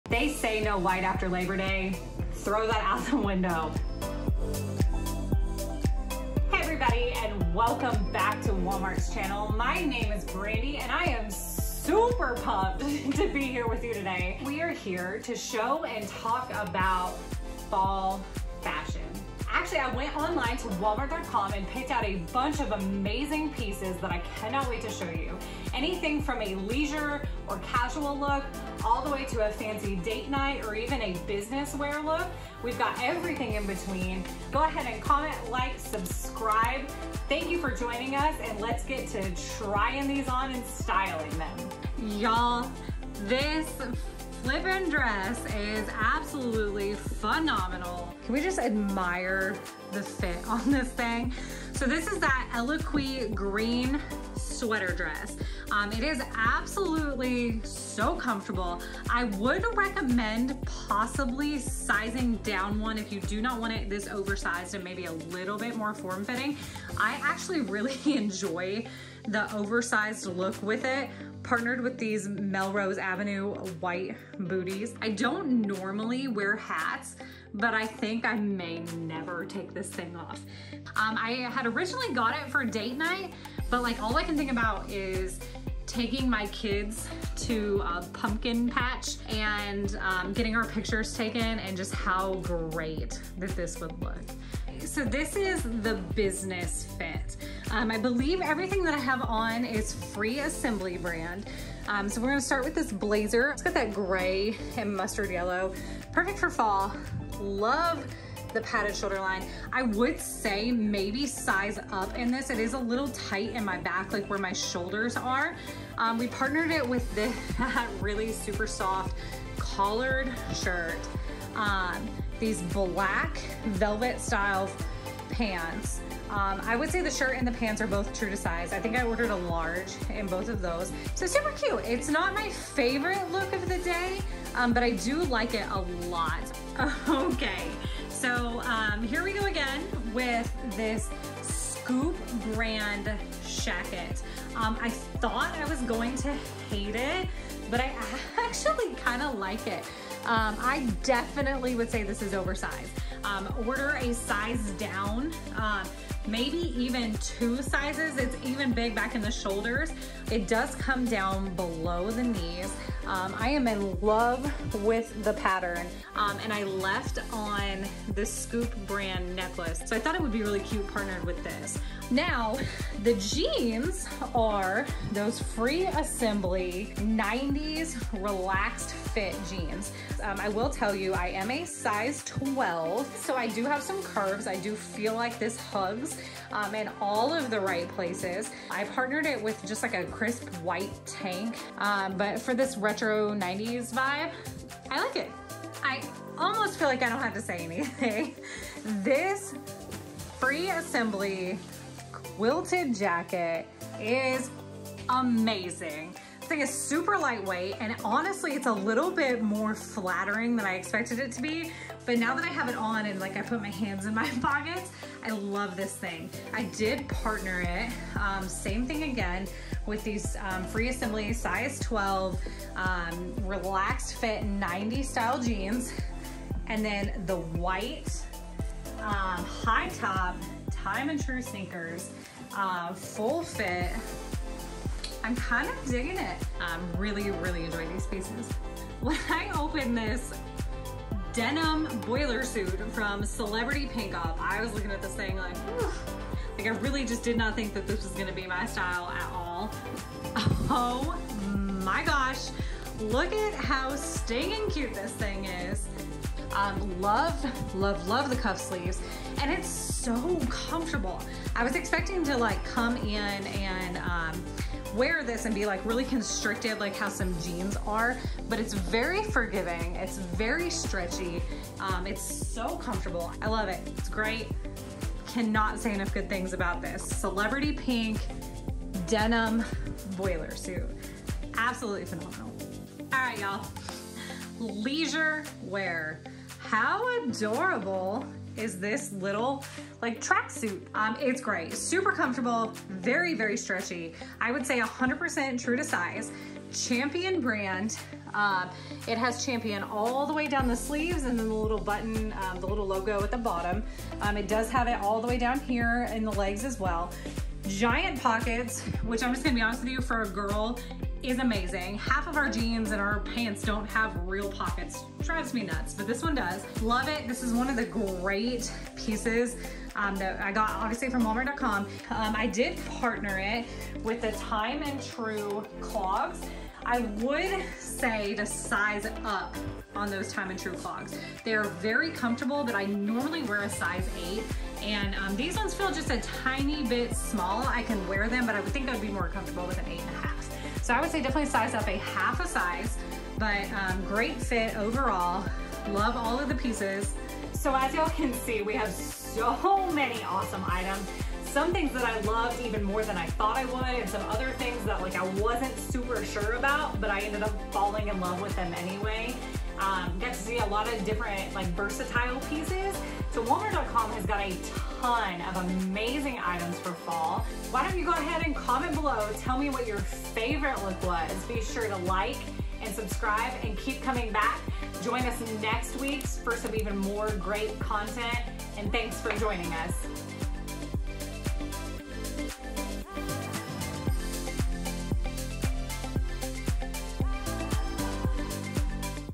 Huge fall fashion haul + how I style
Welcome! In this video Brandi tries on affordable and trending fall fashion finds!

Scoop Womens 14K Gold Flash-Plated Enamel Evil Eye Pendant Necklace Duo, 2-Piece